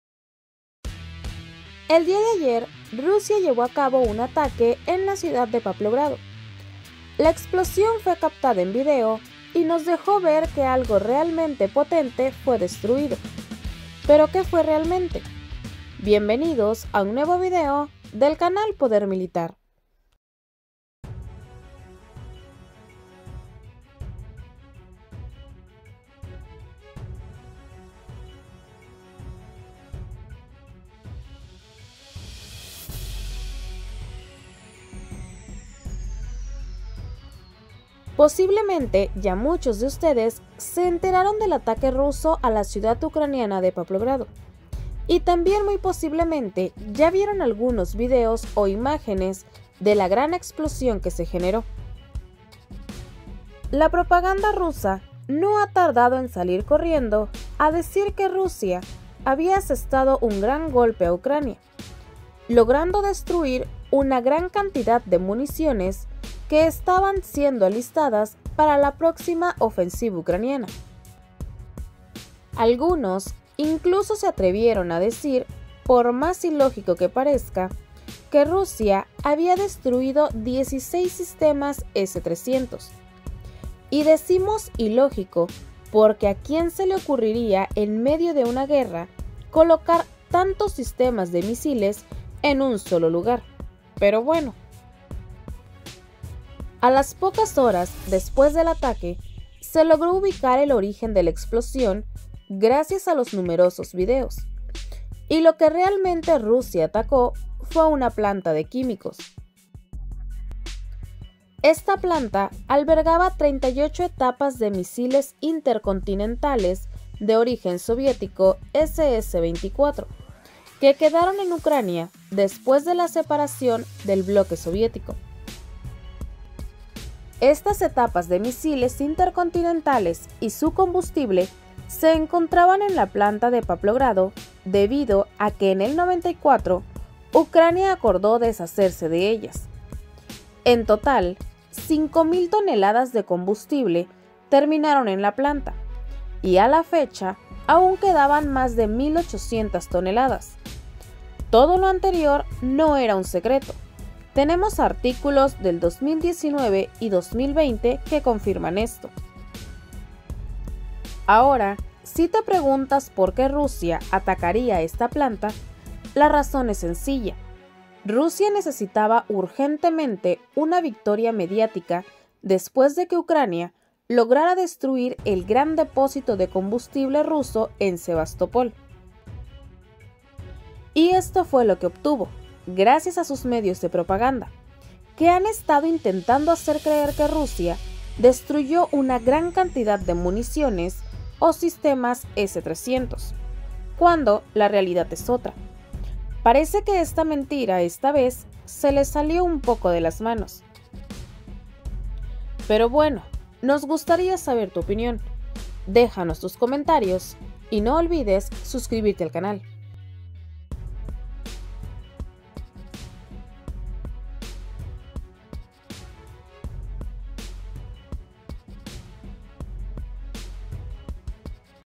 Rusia destruye depósito de misiles intercontinentales
El dia de ayer Rusia llevo acabo un ataque en la ciudad de Pavlograd, la explosion fue captada en video y nos dejo ver que algo realmente potente fue destruido, pero que fue realmente. Bienvenidos a un nuevo video del canal poder militar.

* Equipo:
Narración: Berenice
Edición: JACH
Colaboradores:
- José Luis
- Santiago
- Luis
- Crusp